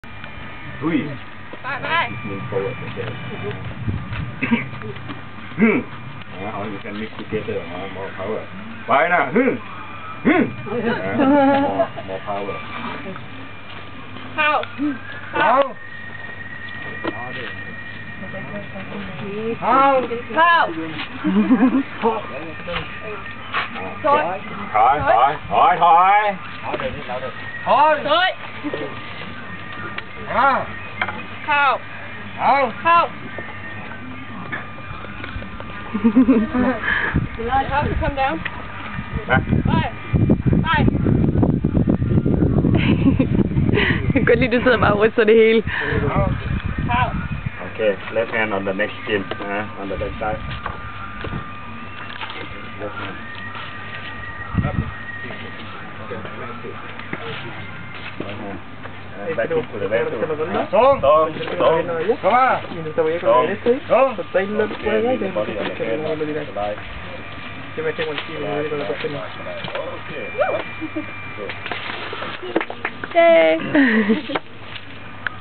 Maevang Elephant Camp - Metten rider rundt
Weekenden d. 3. - 4. marts 2012: Metten og Lisbeths 2 dages mahout træning i Maevang Elephant Camp. Her lærte vi at ride og styre en elefant ved hjælp af thailandske kommandoer som en rigtig mahout!